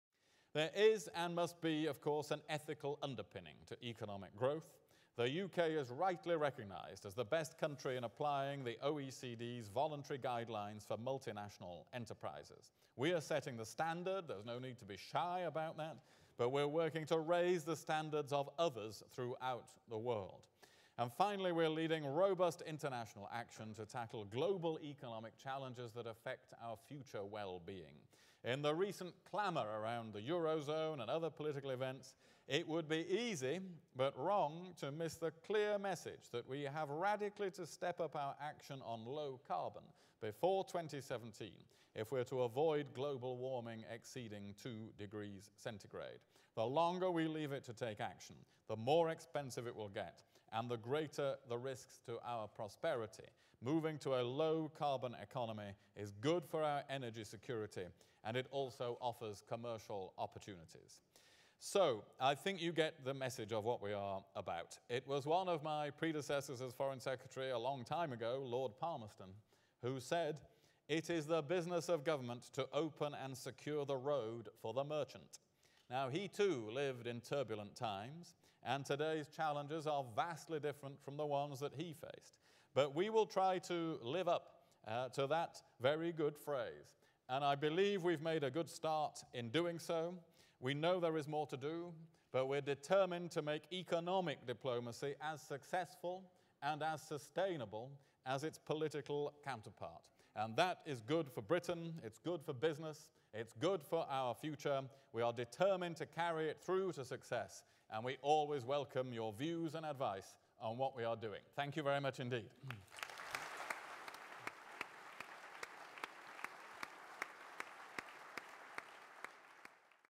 William Hague speaks at the 2011 CBI Annual Conference
Foreign secretary William Hague explains that the government is determined to 'make economic diplomacy as successful and sustainable as its political counterpart'